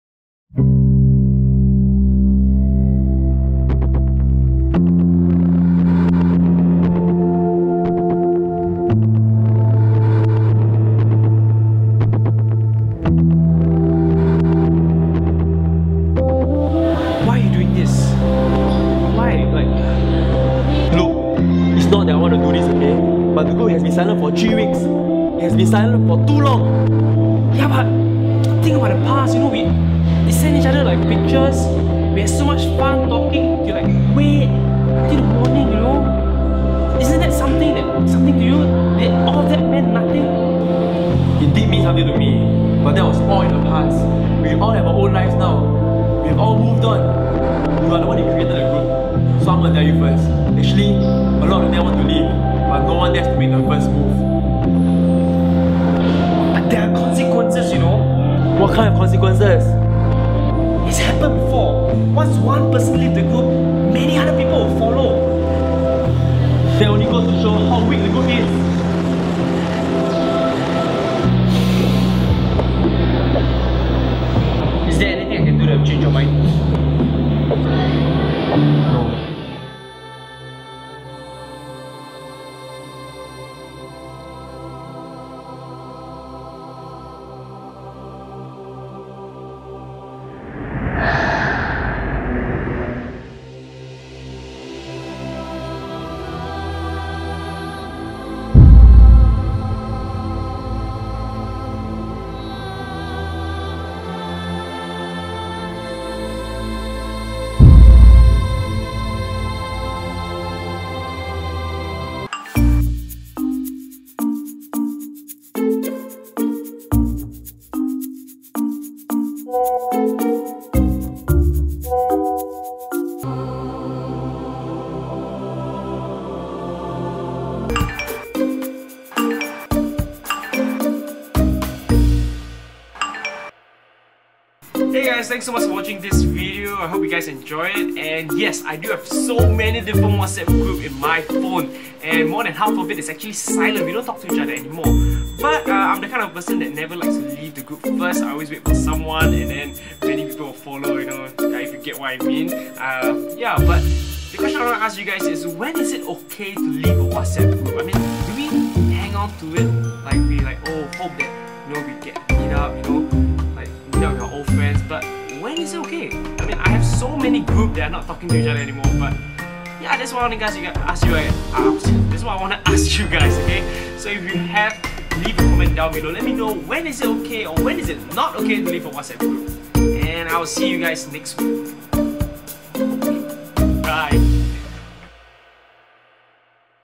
Leaving the group
An emotional scene of leaving of a group.

Special thanks to:
Umehara Ken - Camera Operator
Kevin Tan - Guest Actor